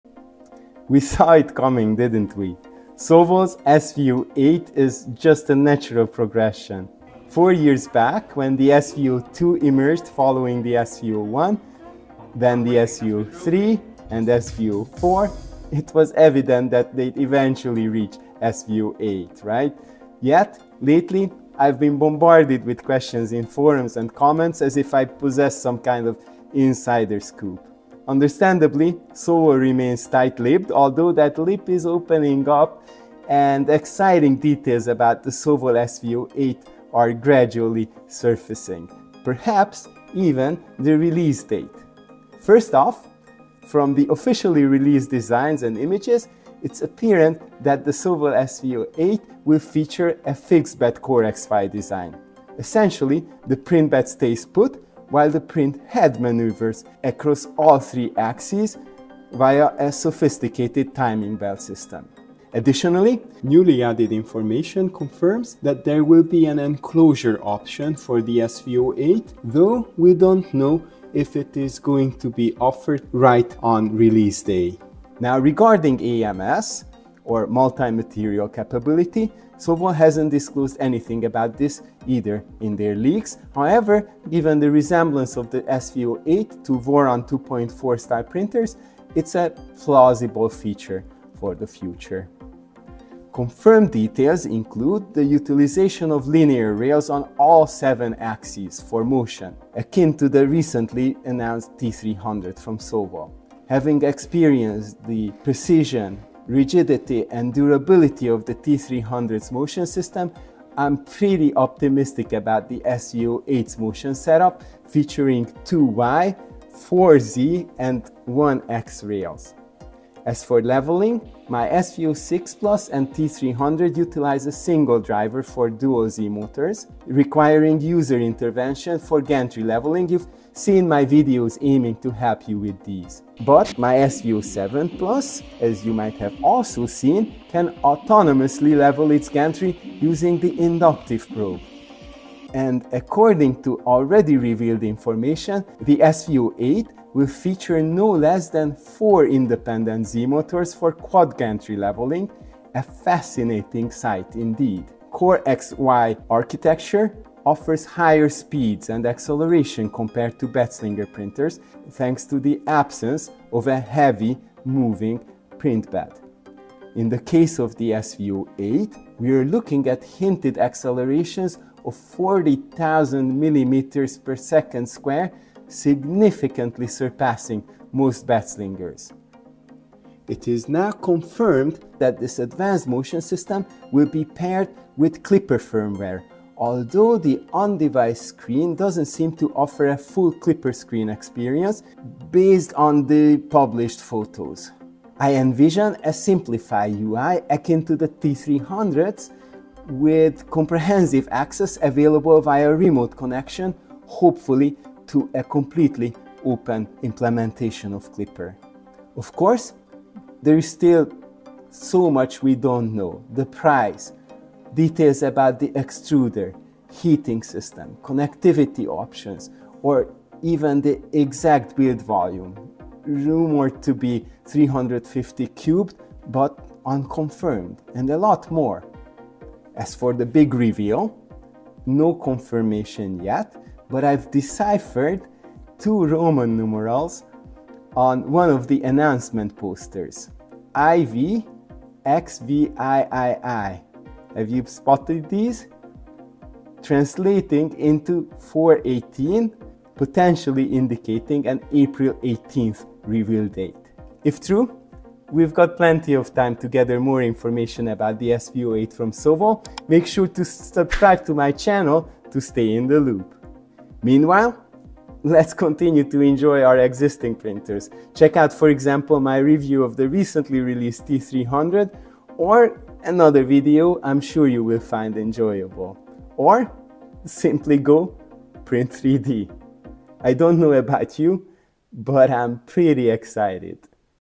Sovol SV08 leaks & rumors - CoreXY? Build volume? Klipper? Voron Clone? Speed? Price? Release day?
► EMMC adapter

Disclamer: I've compiled all the officially released information about the upcoming Sovol SV08 3D printer, as of April 5, 2024.  We will hear more and more details as time progresses, exciting times! 😊 (I personally do not [yet!] have access to this model).

☕ If you find my videos helpful, please say thanks by buying me a coffee